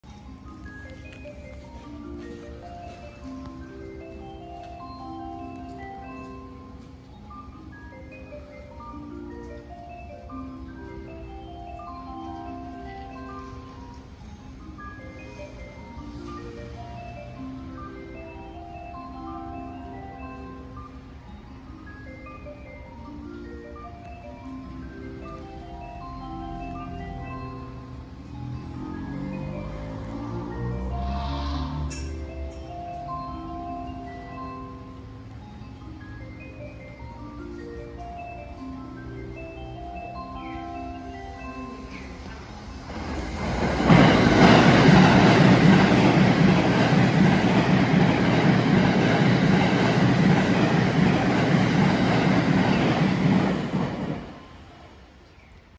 683系4000番台＋2000番台特急サンダーバード21号金沢行き 鯖江駅通過
めっちゃ久しぶりの鉄道動画(投稿し忘れ分)